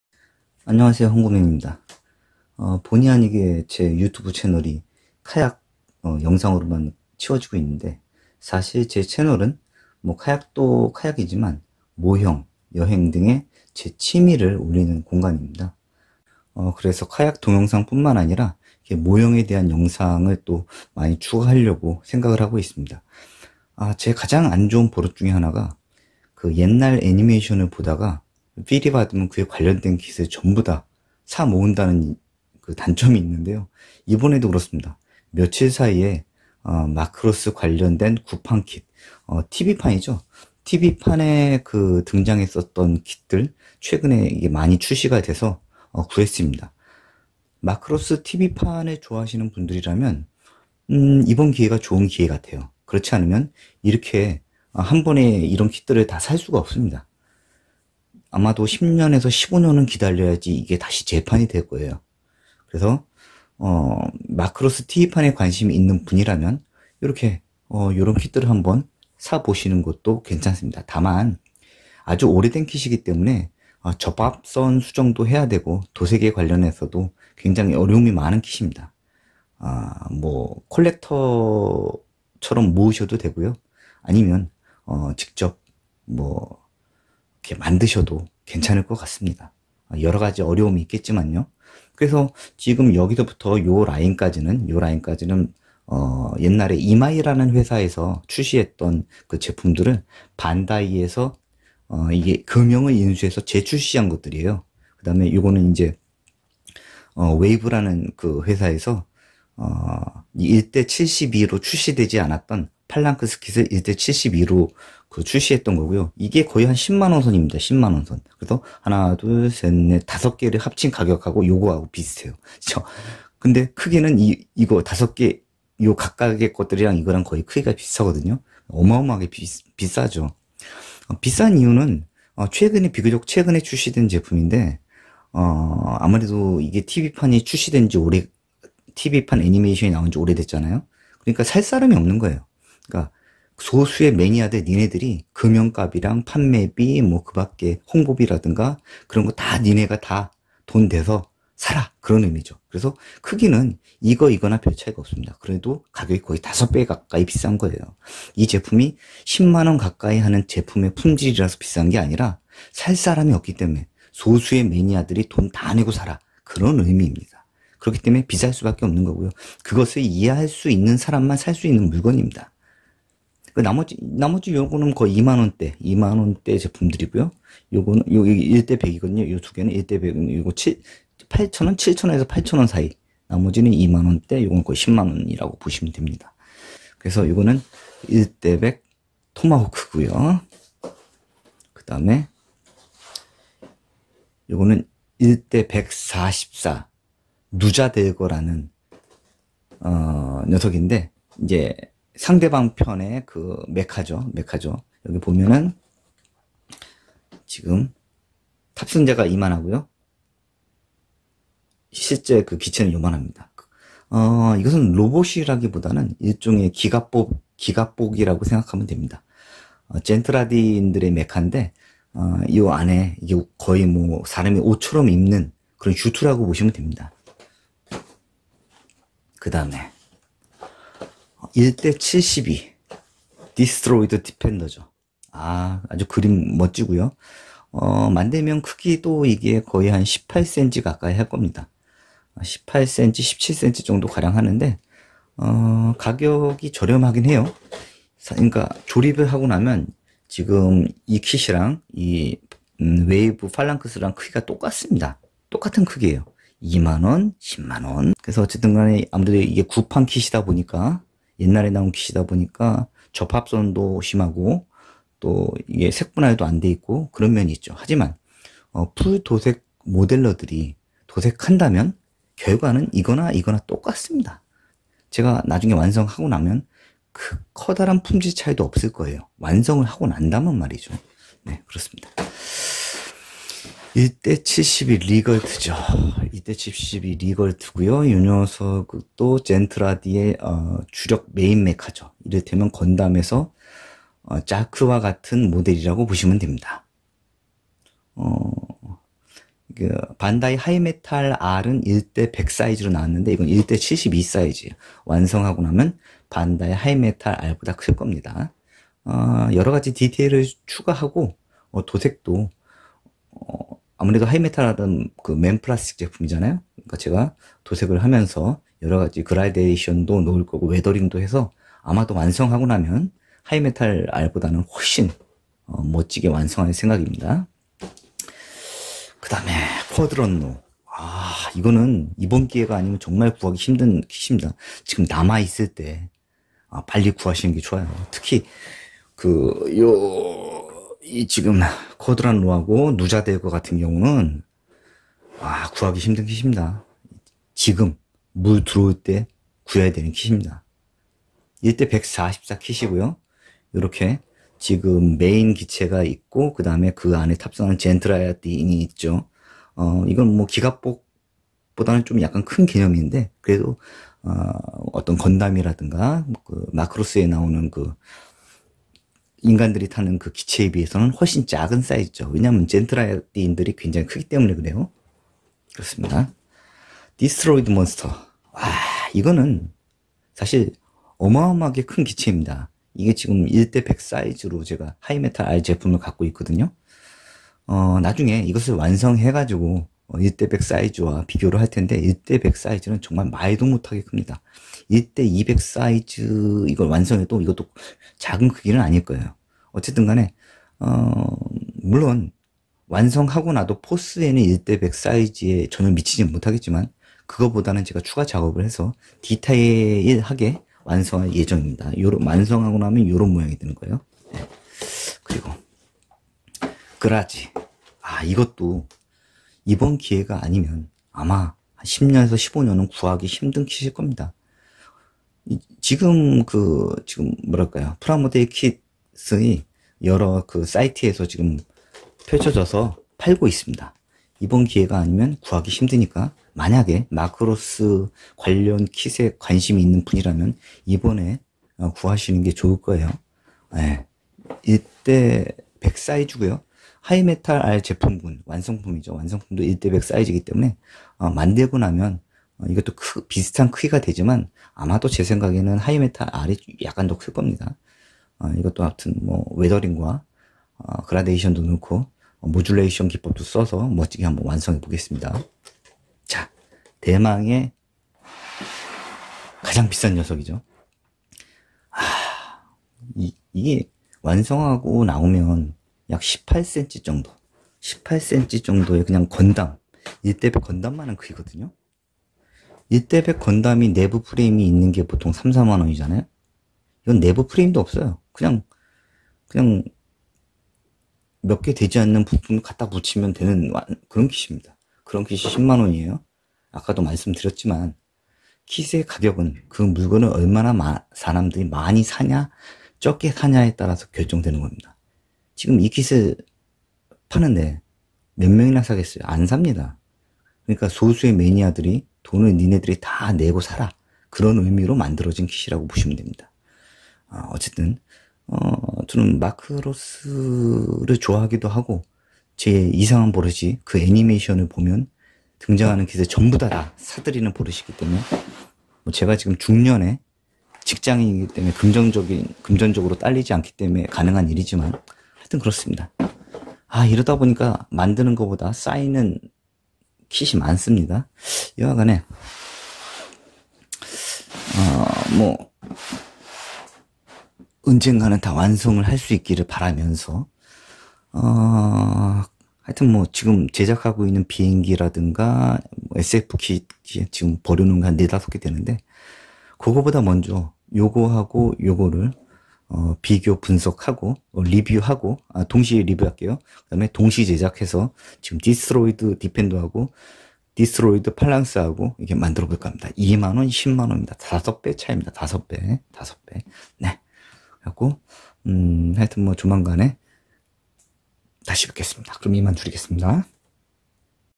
마크로스 TV판 반다이/이마이 구판 킷들과 웨이브 1/72 팔랑스 박스리뷰
마크로스 TV판 반다이/이마이 구판 킷인 1/100 토마호크, 1/144 누자델거, 1/144 쿼드론로, 1/200 디스트로이드 몬스터,  1/72 디스트로이드 디펜더, 1/100 리걸트, 1/100  그라지와 웨이브 1/72 팔랑스의 박스리뷰입니다.